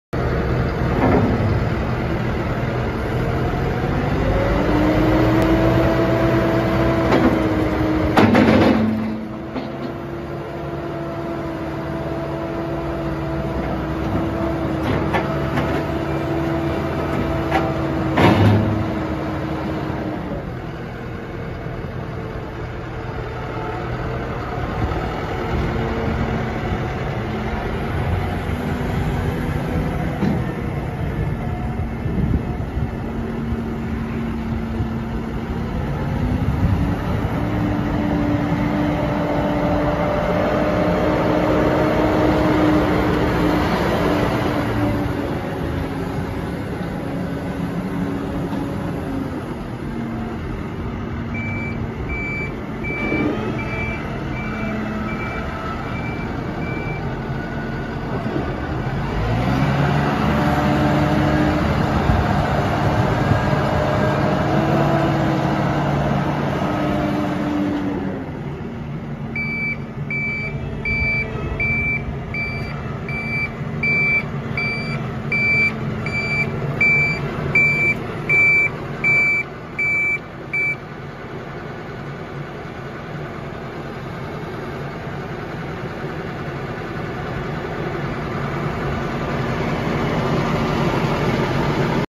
2018 CASE IH MAXXUM 115MC TRACTOR WITH LOADER For Sale
Cab, 511 Hours, Heat & Air, 4X4, LHR, Case IH L105 Loader w/ GP Bucket, 24F/24R Powershift Transmission, 115 HP FPT Diesel, 3PT, 540/1000 PTO, Drawbar, 3 Rear Remotes, 4630 LB Lift Capacity, Operating Weight: 13500 LBS, Located at McGrew Equipment Company (Seven Valleys, PA)